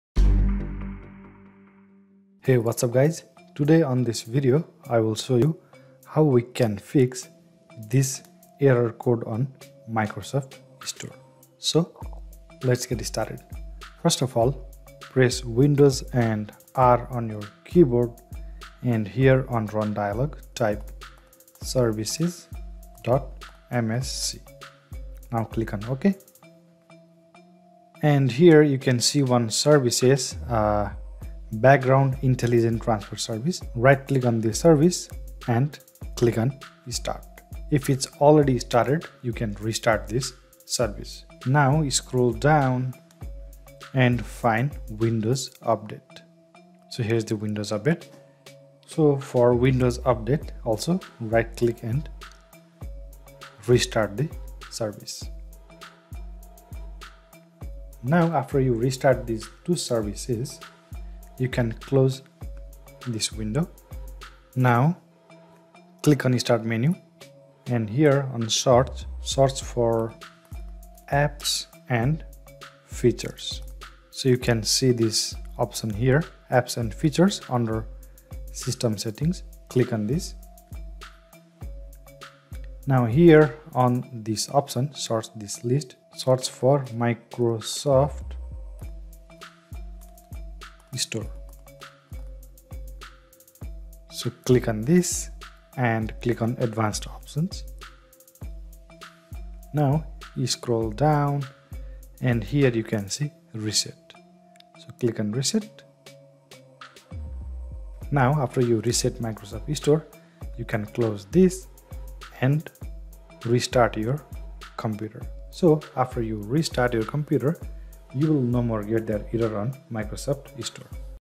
How To Fix Microsoft Store Error 0x80070424 and Windows Update Error 0x80070424
Are you getting error 0x80070424 on windows update or Microsoft store? On this video I will show you how we can fix error 0x80070424 on both windows update and Microsoft store. This fix will work for both Microsoft store and windows update error.
If you follow all the steps on this video correctly, then you will be able to fix those error codes on Microsoft Store and Windows update and you will no longer get error 0x80070424.